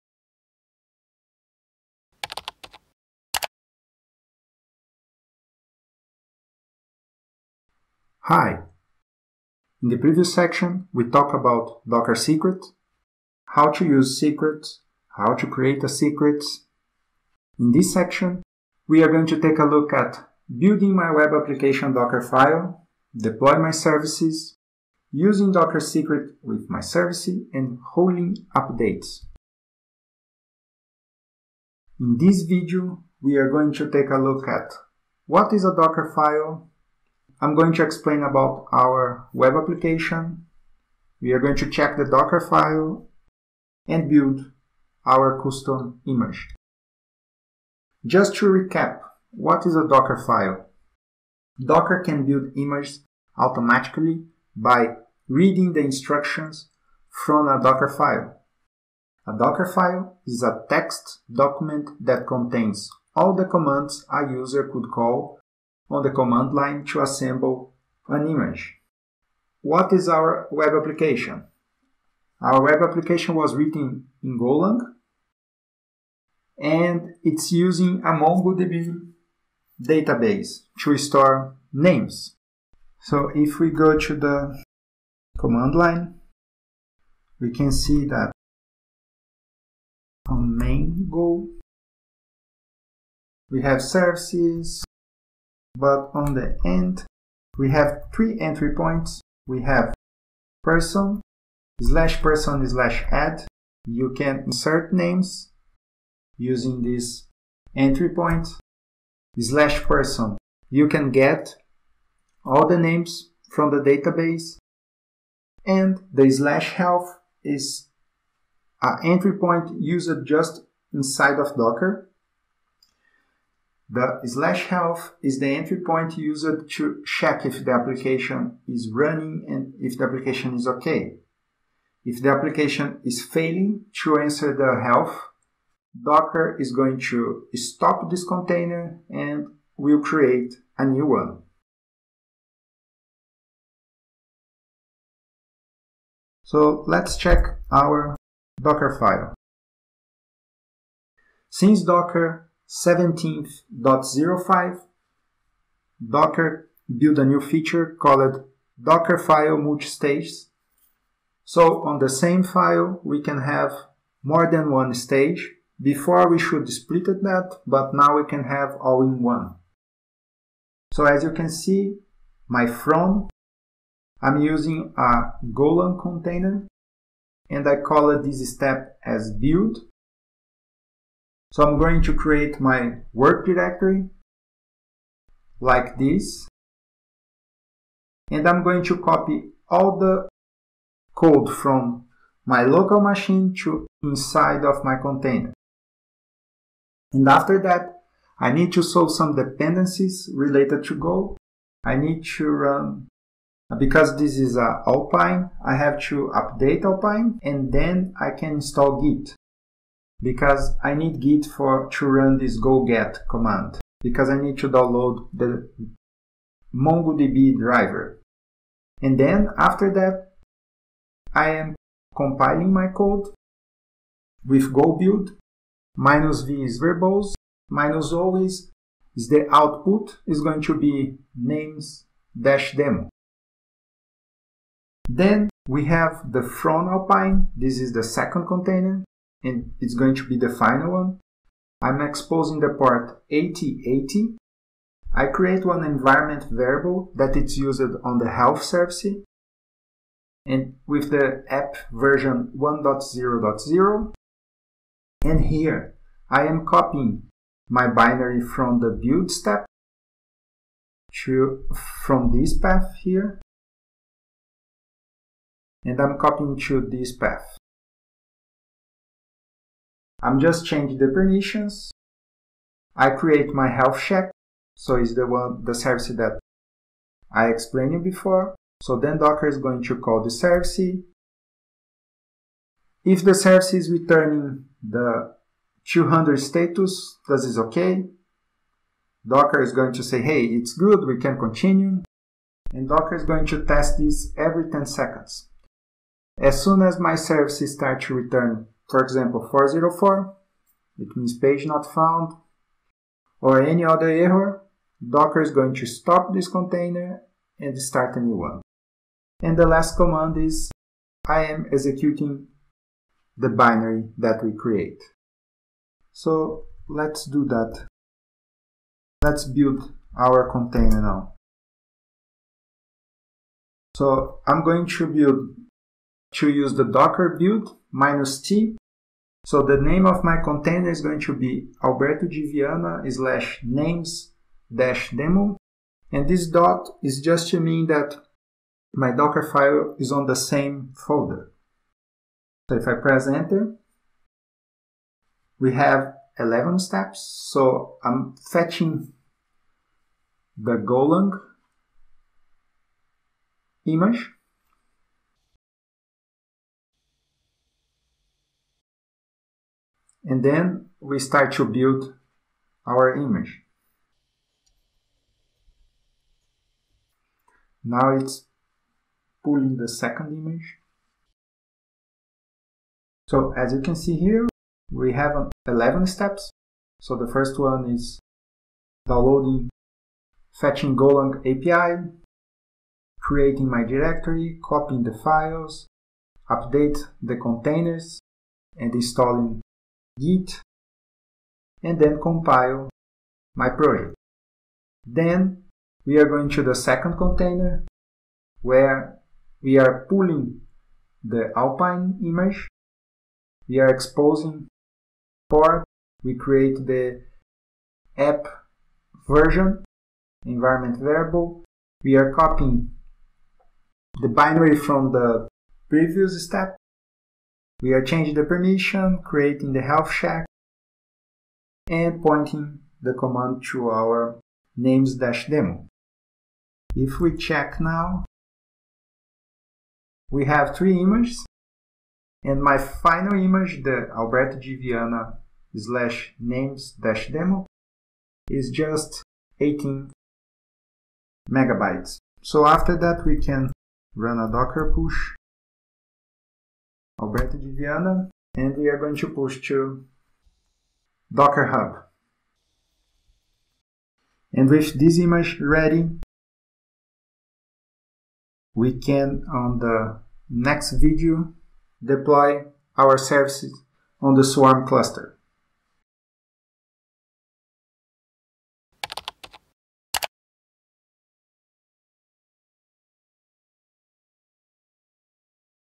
Docker Swarm : Build My Web Application Dockerfile | packtpub.com
This video explains what is a Dockerfile, introduce the application and build a custom image.
• Explain what is a Dockerfile
• Show the application Dockerfile
• Build a custom image